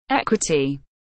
How to pronounce Equity / Equity pronunciation
Raccoon teaches you how to pronounce Equity . Raccoon vous apprends a prononcer Equity . Como pronunciar Equity